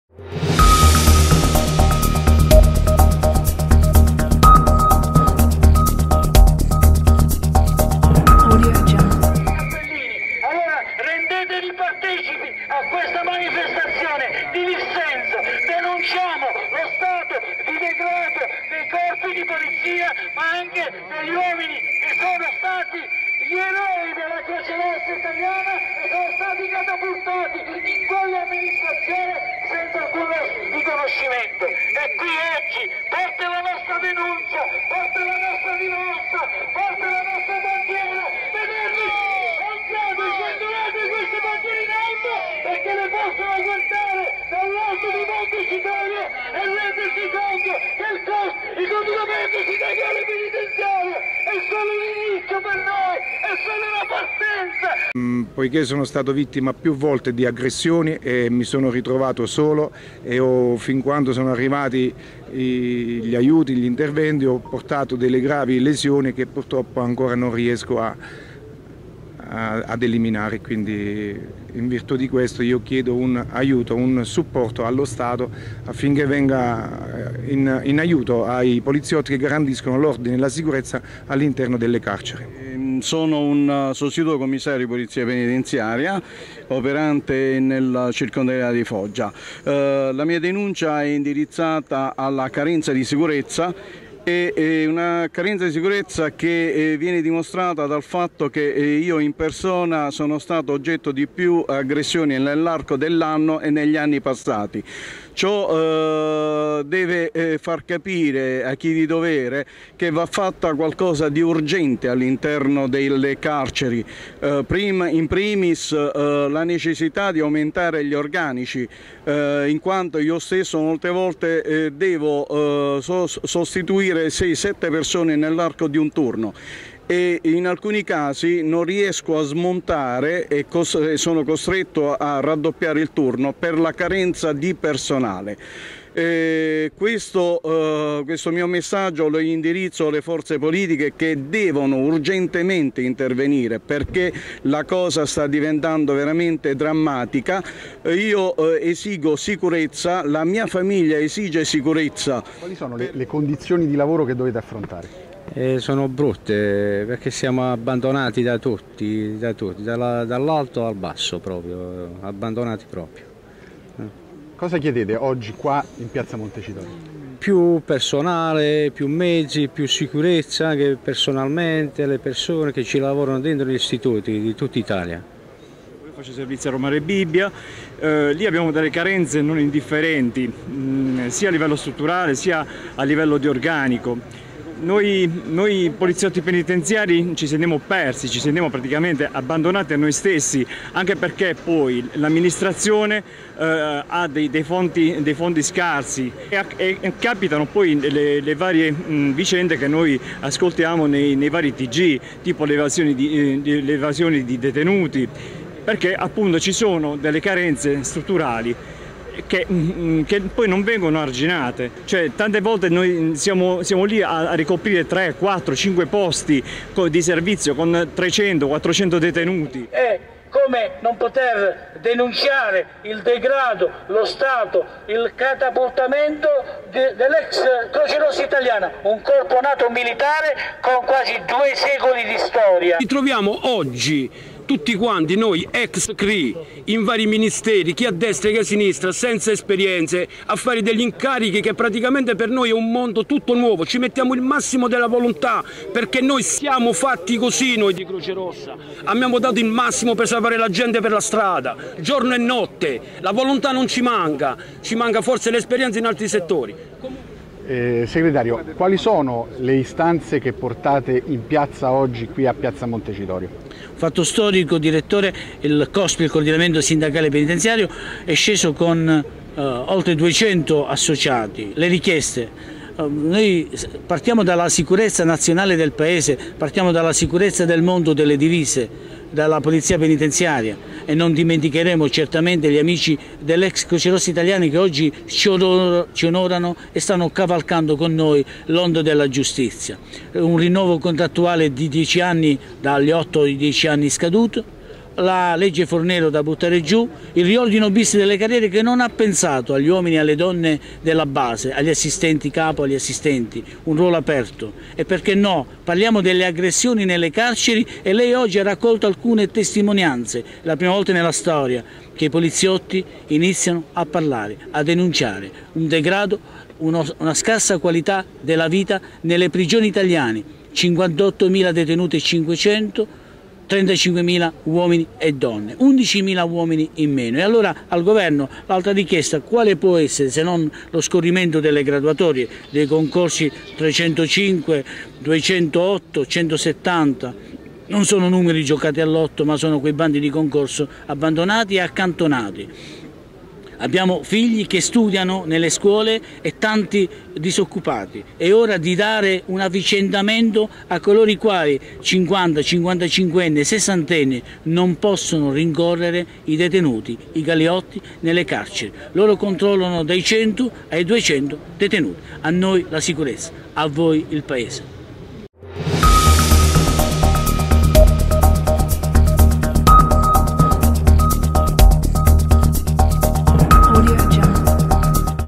Roma, 13 dicembre 2017 Piazza Montecitorio, in piazza il sindacato CO.S.P.
In piazza gli agenti della polizia penitenziaria e i dipendenti della funzione pubblica, per protestare contro le condizioni di lavoro e le aggressioni che sempre più spesso subiscono gli operatori in servizio. La carenza endemica di personale e i tagli lineari al budget del sistema penitenziario contribuiscono ad alimentare un crescente clima di sfiducia e di incertezza nel quale vivono gli operatori carcerari. Il segretario generale del CO.S.P Domenico Mastrulli spiega quali sono le rivendicazioni del sindacato.